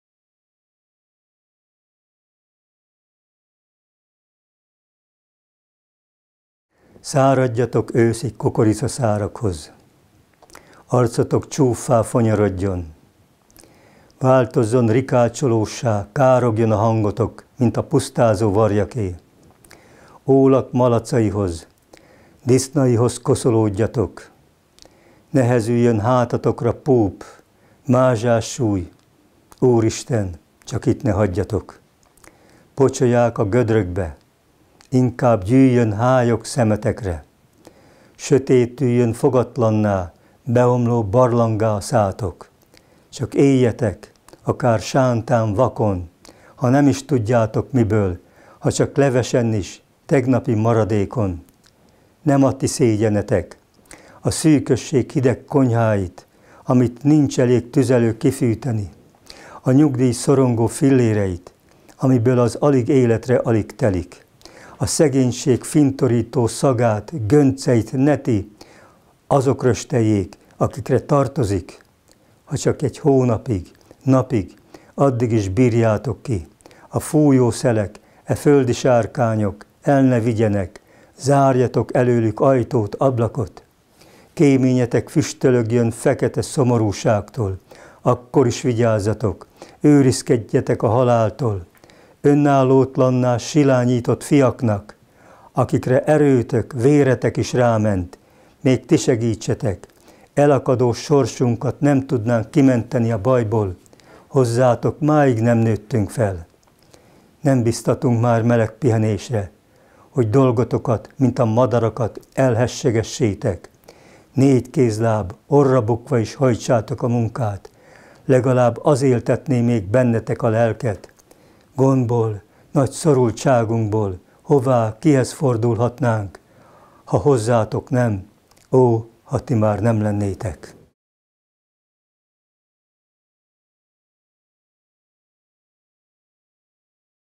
Serfőző Simon: Öregek - a költő előadásában
A felvétel 2015. januárjában készült Zagyvarékason.
MMA, 2016